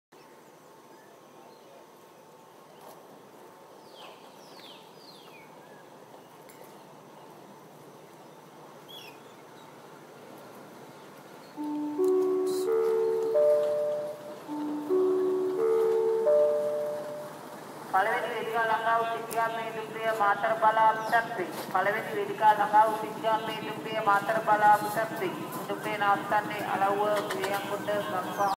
SLR Class M9 872 (Alstom Diesel)
Taken: At Polgahawela, Sri Lanka
Train : Kandy-Mathara Express
Locomotive : Class M9 872 (Four Stroke - Alstom Diesel)